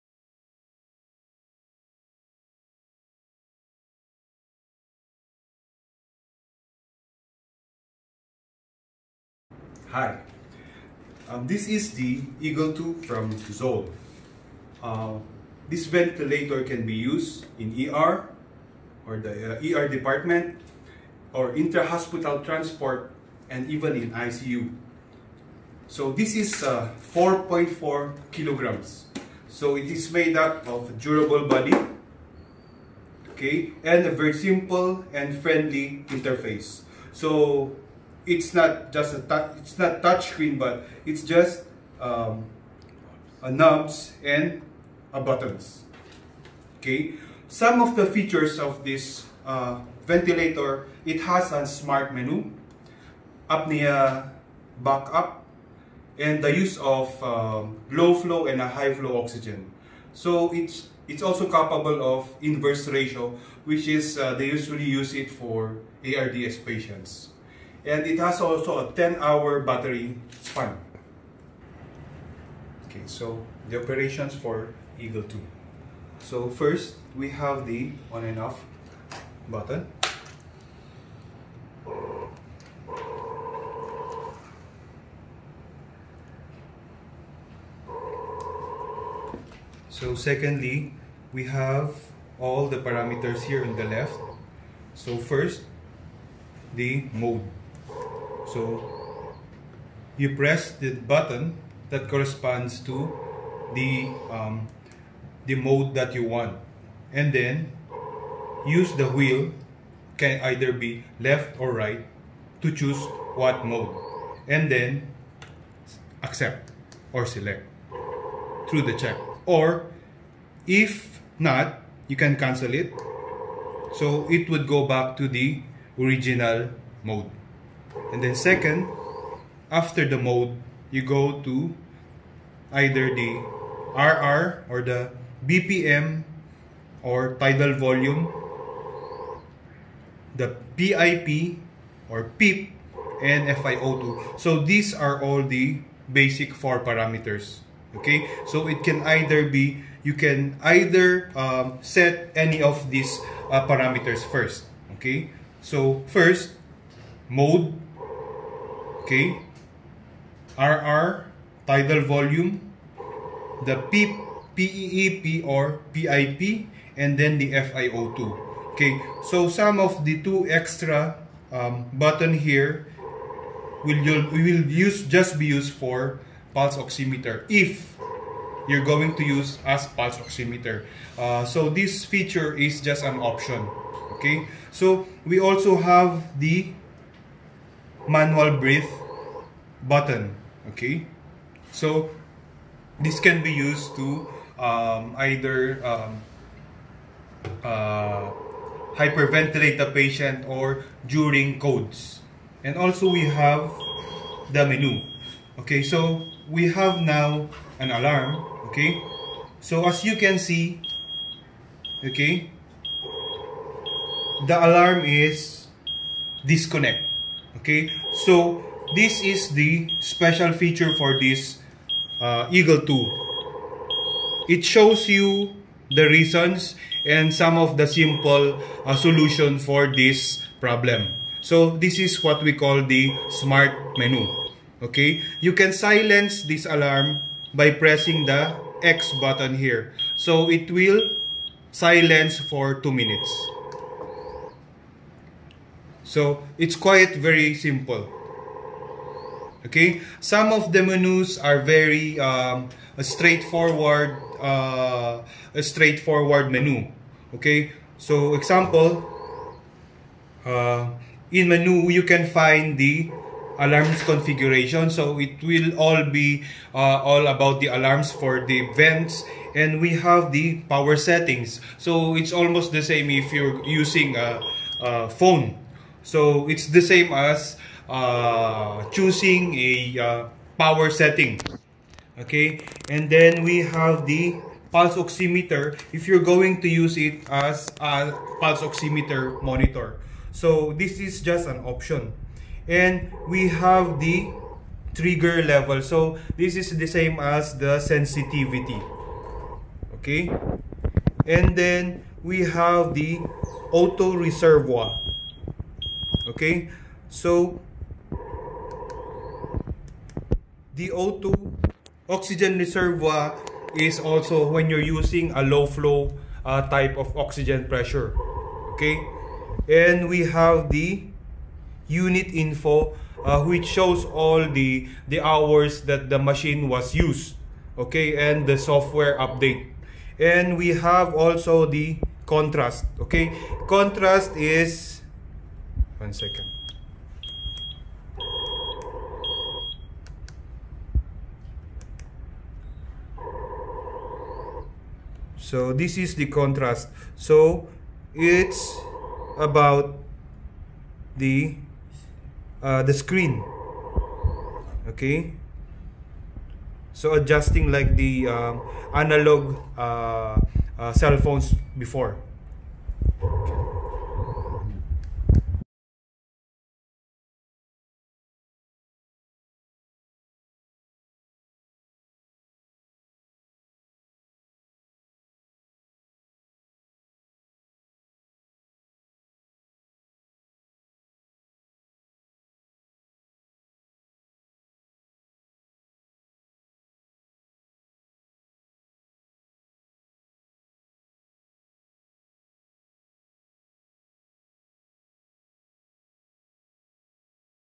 How to use ZOLL EAGLE II ventilator
Short tutorial demonstration video on the functionality of the ZOLL EAGLE II ventilator.